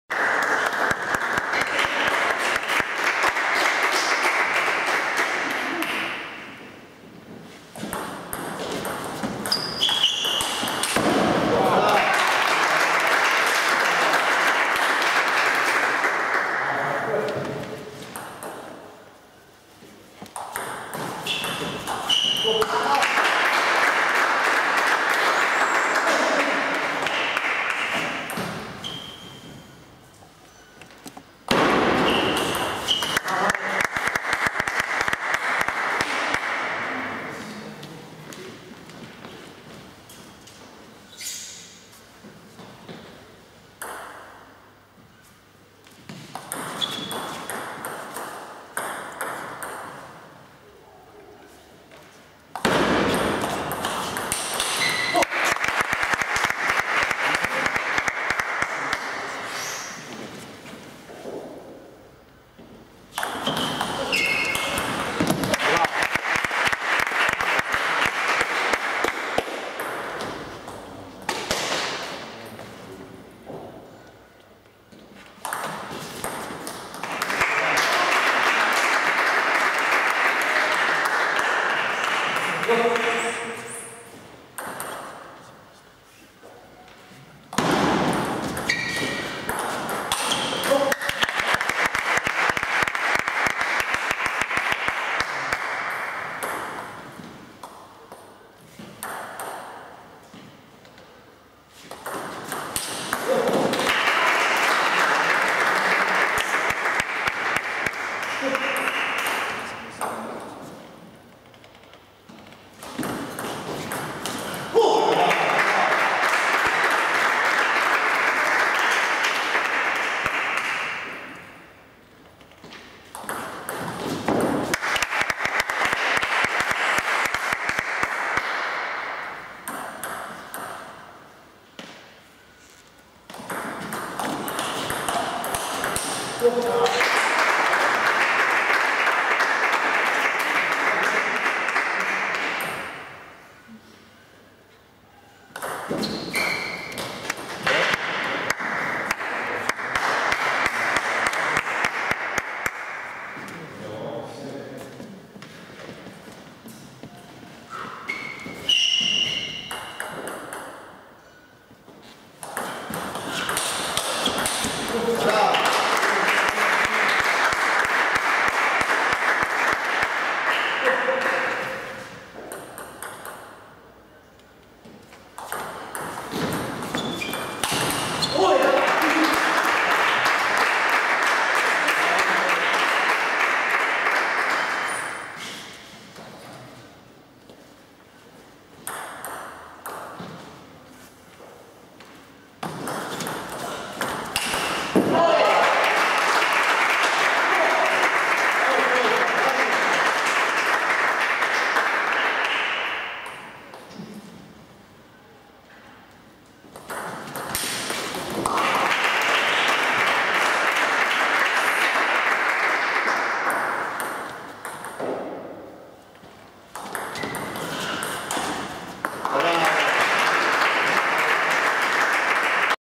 Mijatović Danijel - Dobrić Raymond 1. set (11 : 6)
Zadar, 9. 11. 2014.
Donat II - Brovinje  4 : 1
Mijatović Danijel - Dobrić Raymond 3 : 0 (6, 10, 7)